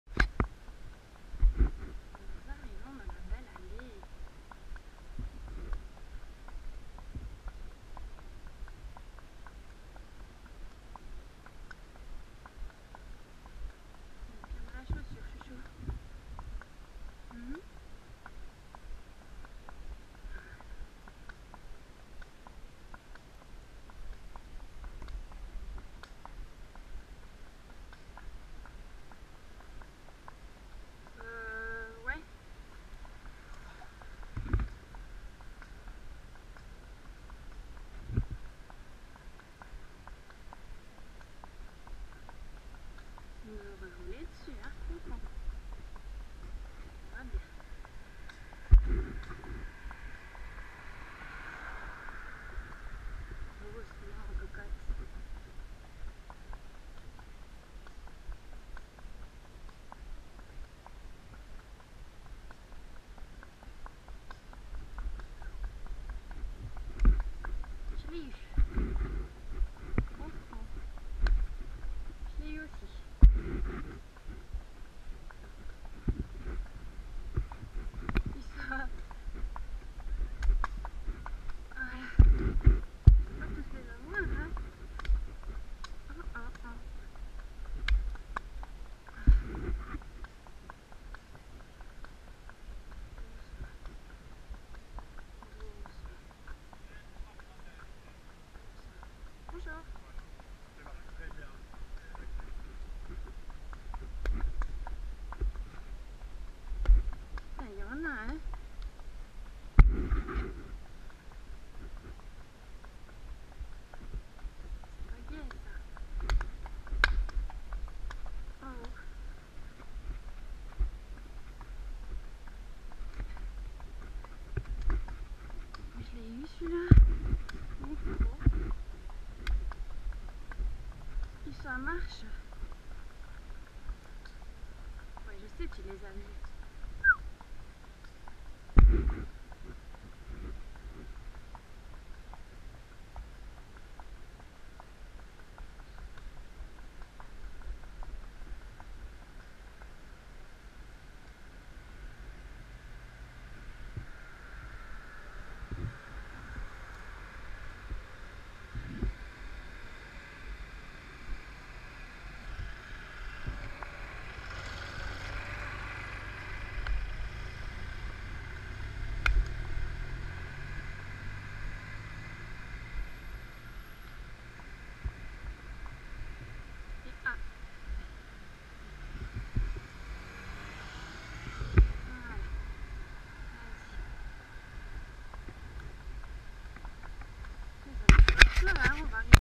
Balade montée 4/07/16
Première balade montée en tete a tete depuis le déménagement, usoa a bien géré. Une horde de taons nous a bien embetées, ce qui la rendait assez allante ...